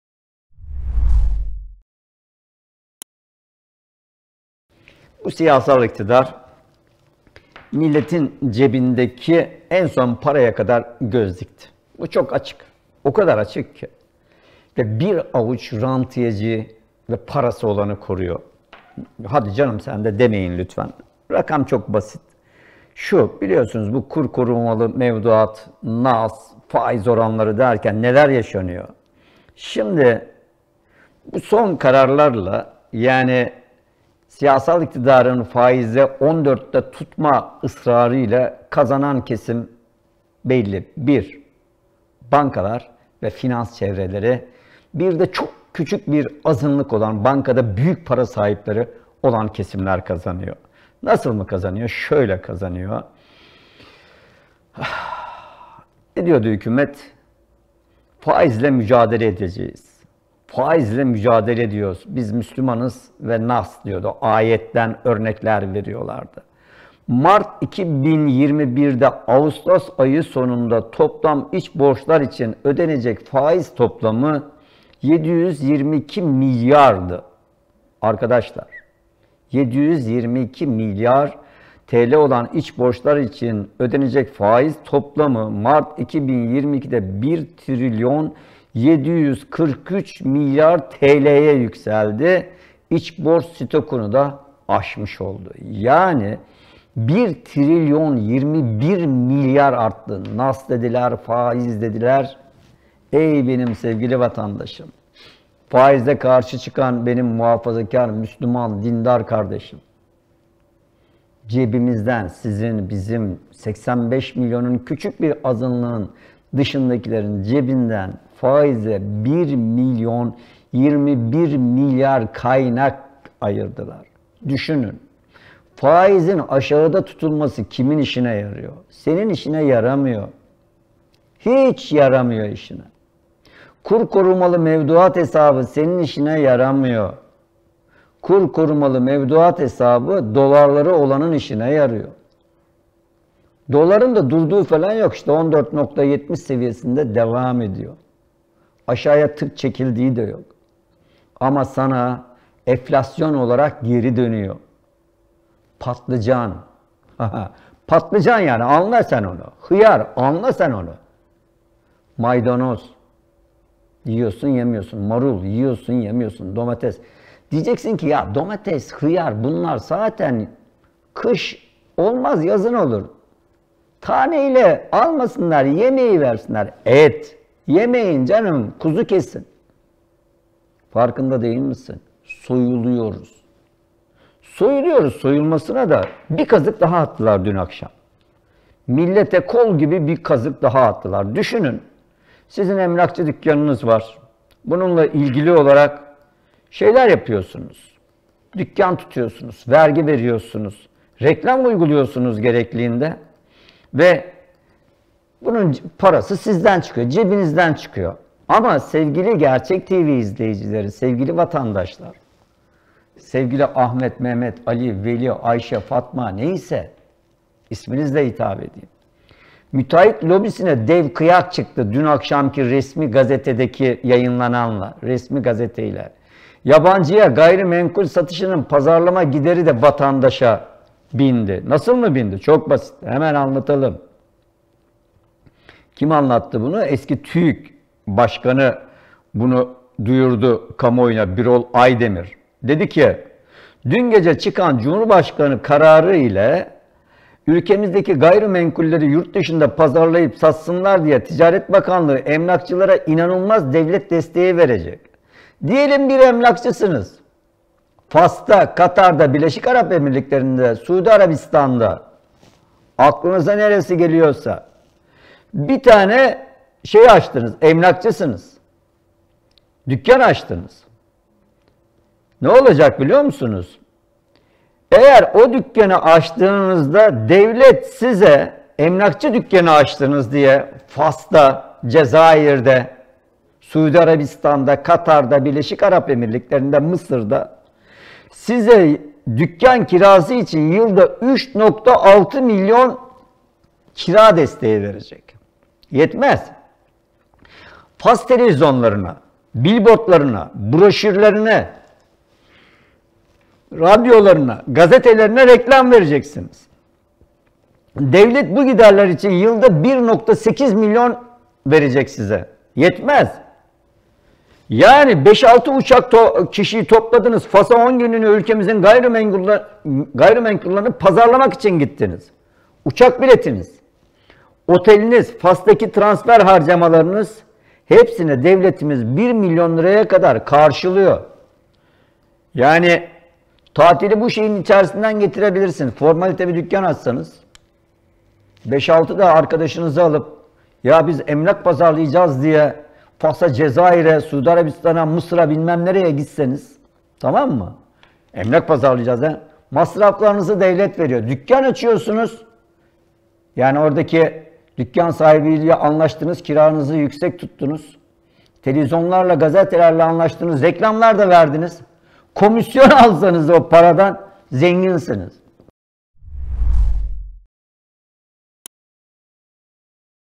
MÜTEAHHİT LOBİSİNİN PARASI DA VATANDAŞTAN ÇIKACAK - ALİ TARAKCI
Ali Tarakcı ile Gündem'de;
- Faizin 14’te tutulmasının sebebi belli
- Müteahhit lobisine dev kıyak!